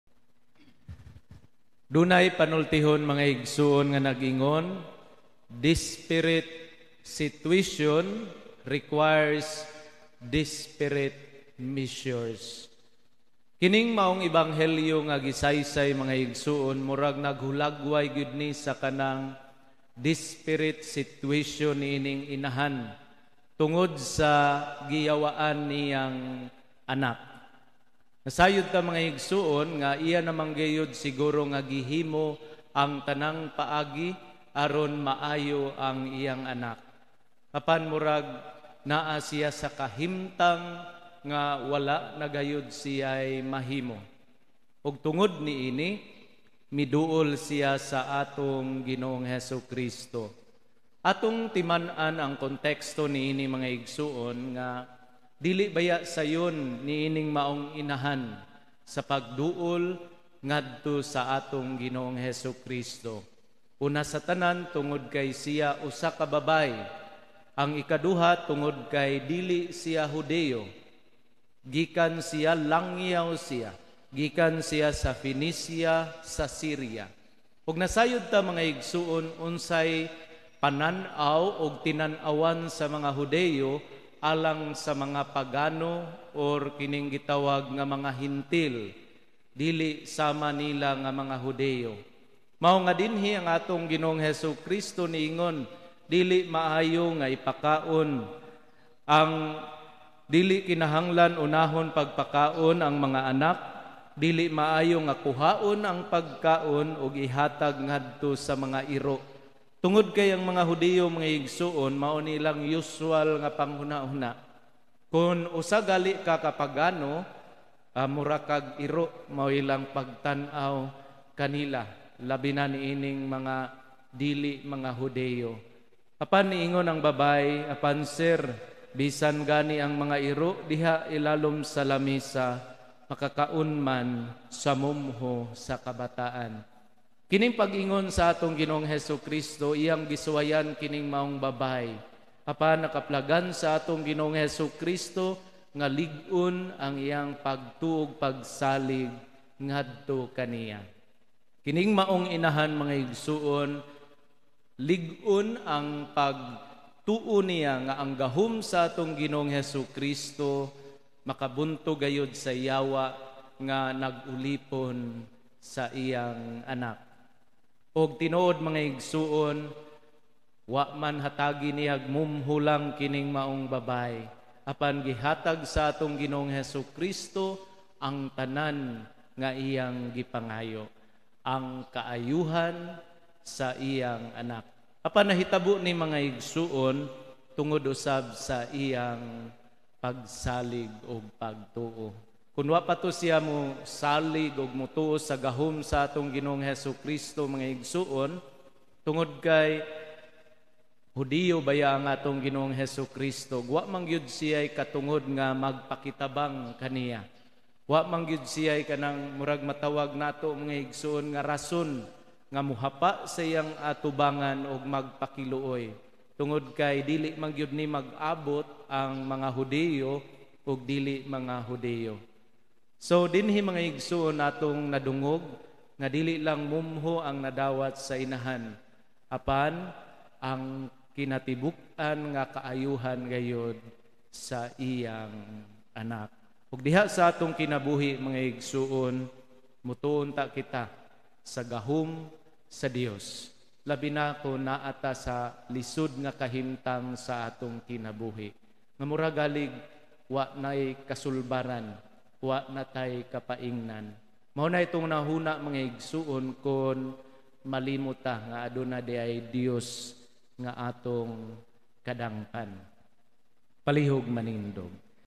Homily: IKA-LIMA NGA SEMANA SULOD SA TUIG Rev. Fr. Arnel Ponce
IKA -LIMA NGA SEMANA SULOD SA TUIG
Mass Presider: Rev. Fr. Arnel Ponce
San  Vicente Ferrer Parish Liloan,Cebu
Thursday Febuary 10,2022